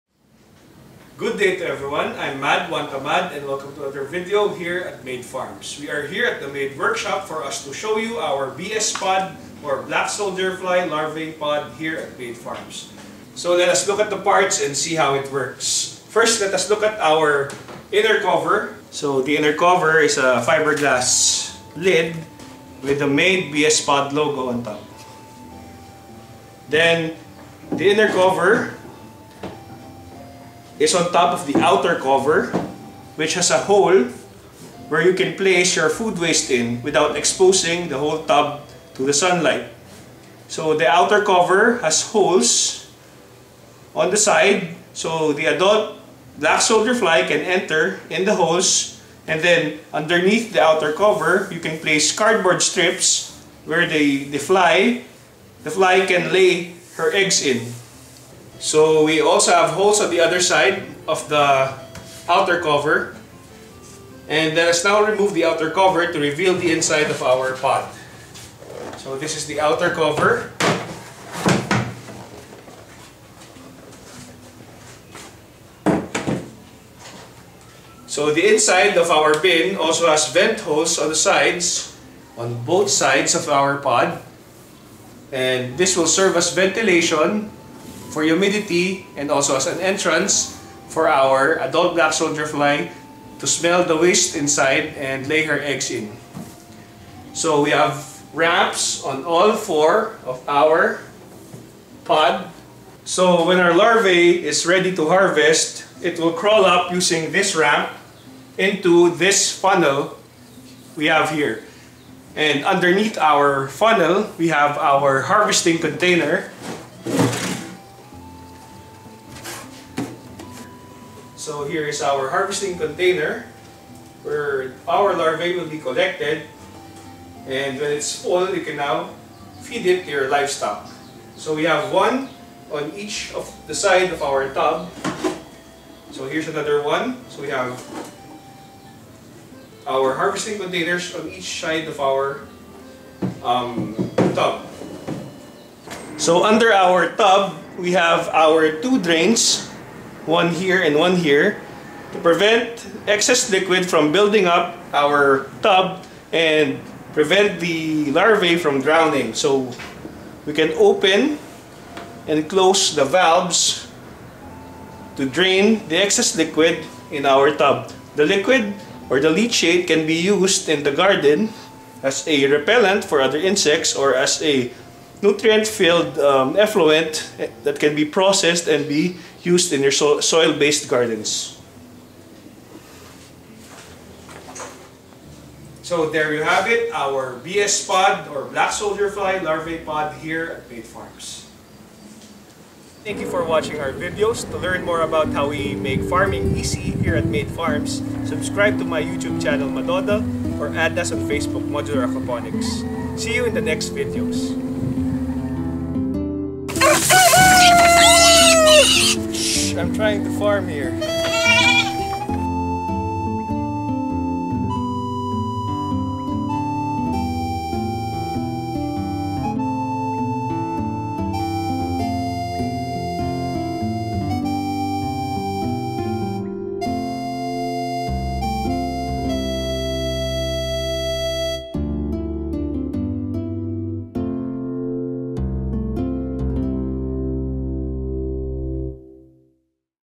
Black Soldier Fly Larvae Composting, BS POD Design by MADE
BSFL Composting

BS POD Design by MADE for Black Soldier Fly Larvae Production

Help fund our farming experiments:
For bitcoin: 183nauCvgMADbSpR1dFDbwK7bKfVeXVfpp

Modular Aquaponics Design and Equipment